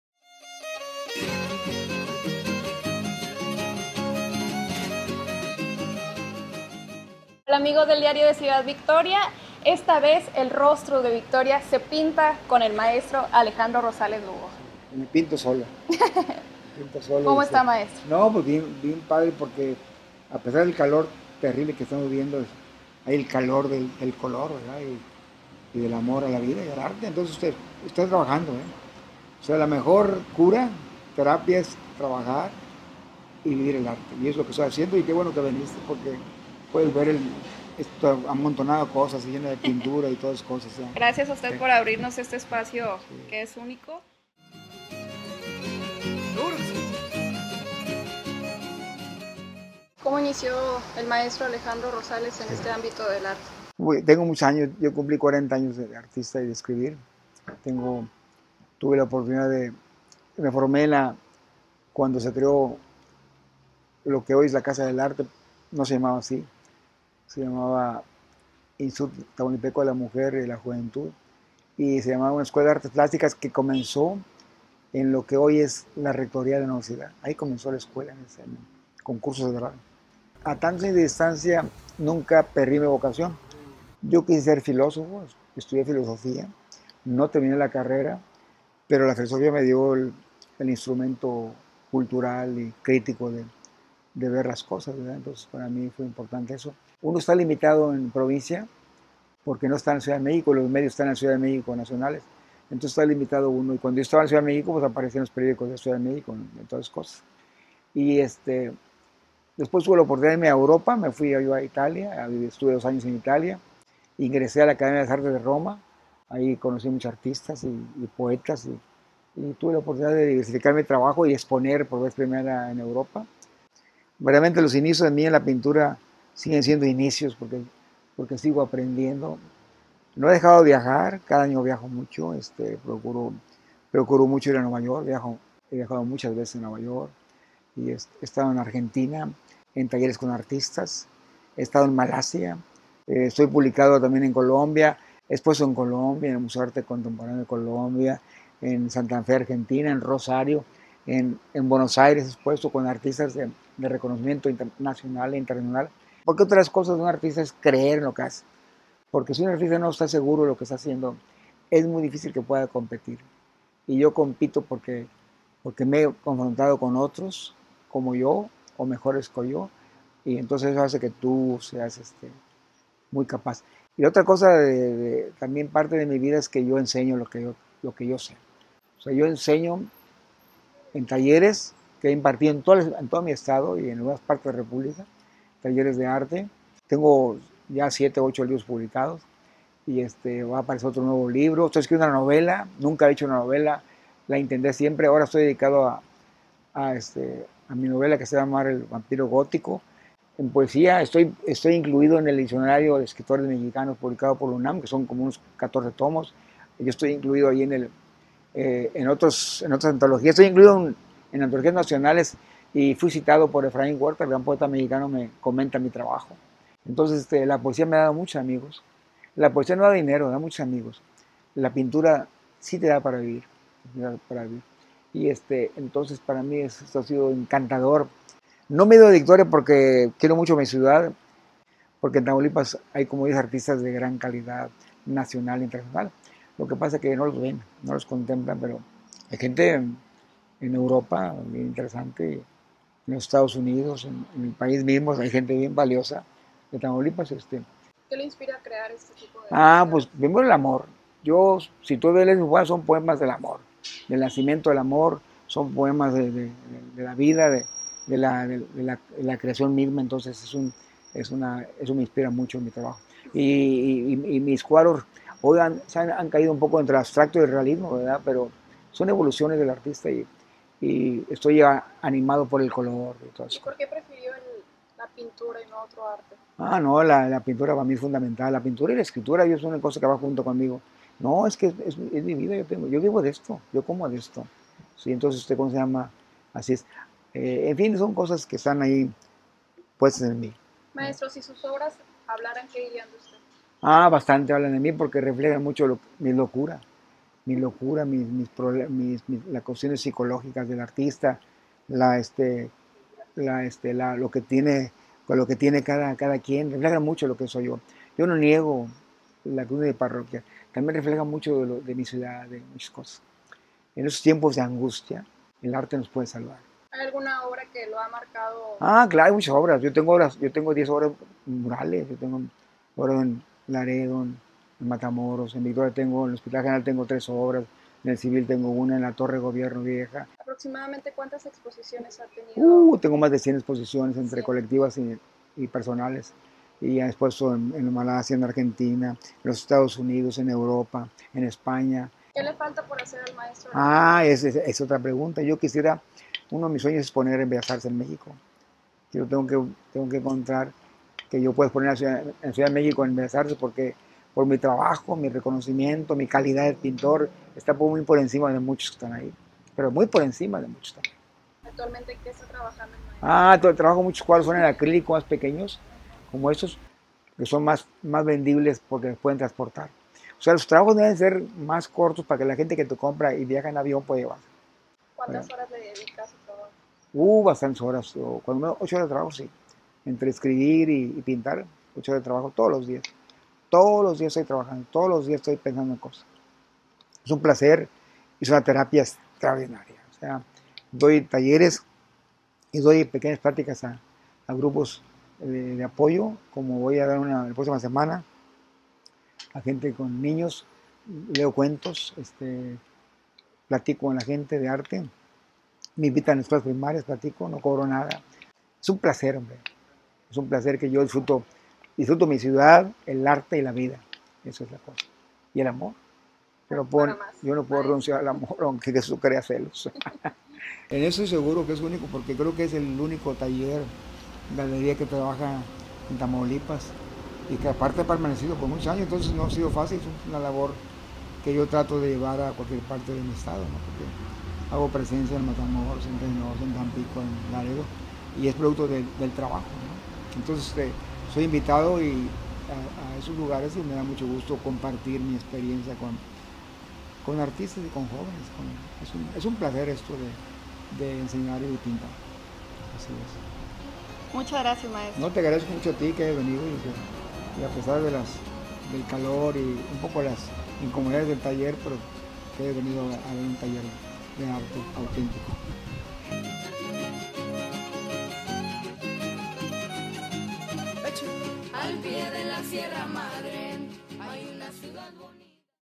Una vida llena de arte
Alejandro Rosales Lugo nos comparte cómo lleva a cabo su pasión y lo que ha sido su inspiración a lo largo de su carrera. No te lo pierdas.